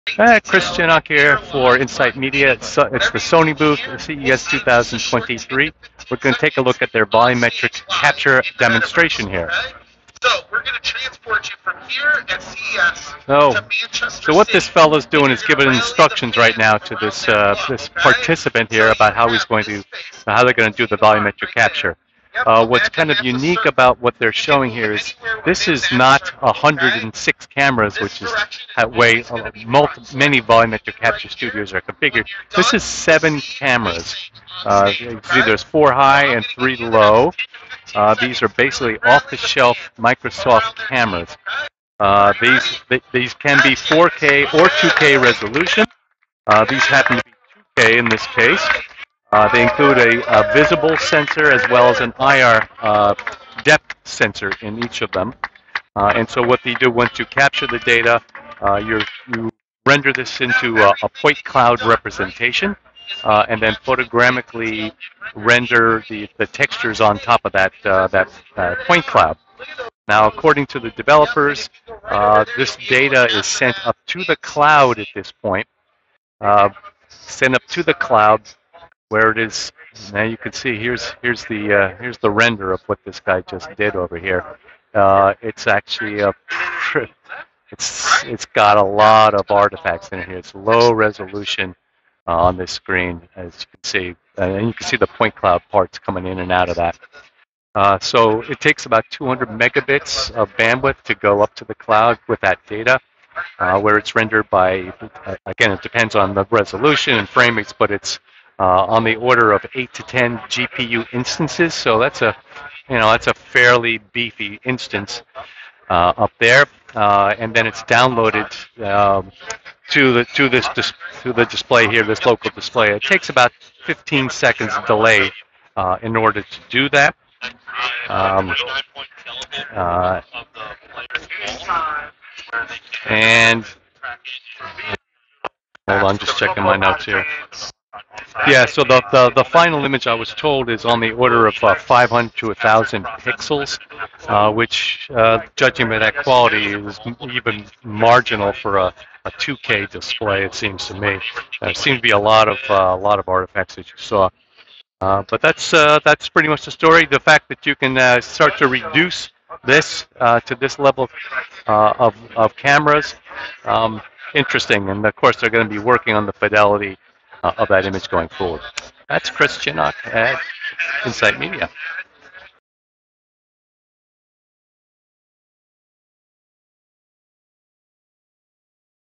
CES 2023 Sony Demonstrates Volumetric Capture with only 7 Cameras
Volumetric capture studios often feature a 100 2K or 4K cameras to create 3D video assets. But there is a need for more mobile capture studios and less expensive and complex ones as well. Sony demonstrated one answer to this need with a 7 camera capture set up.

In their demo, they asked volunteers to do a brief capture sequence. The cameras can be 4K or 2K, but 2K in the CES demo., They are off the shelf Microsoft cameras that include a depth sensor to capture the point cloud data.

In the Sony workflow, the locally captured data is send to the cloud, using the seven 2K cameras, this requires a bandwidth of about 200Mbps, so professional grade and similar to a broadcast contribution signal. Rendering in dependent upon the number of camewras, resolutoin and frame rate, but for Sony's use cases, they noted that 8-10 GPU instances are needed, which is a heavy load. The depth data is used to creat a 3D point cloud model with the video textured on top and rendered out as a video file.

The volumetric person showed a lot of visible artifacts from the point cloud render and low fidelity of the texturing. I was told such figures are rendered in 500 to 100 vertical pixels, to the final video frame is most likely 1920x1080. A 15 second delay is needed for the entire process.